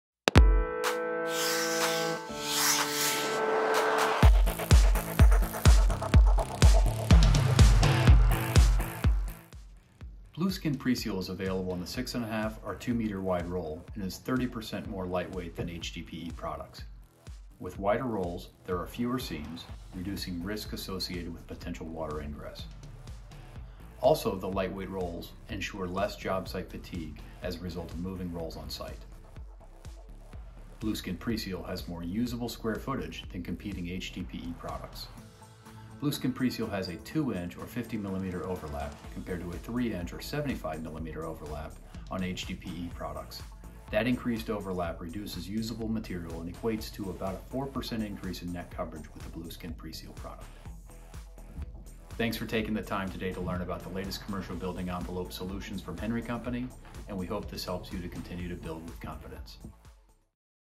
Henry Blueskin PreSeal Features and Benefits
Blueskin PreSeal™ is a pre-applied, fully, and permanently bonded composite sheet membrane for commercial foundation waterproofing applications. Features & benefits include:

• Fully adheres to substrates eliminating water migration.
• Easy installation with self-adhering laps and splices (no tools or torches required)
• Wider roll width results in fewer seams, reducing risk of water ingress
• Better cold weather flexibility than polyethylene based membranes
• Barrier to water-vapor and radon gas permeation
• High chemical resistance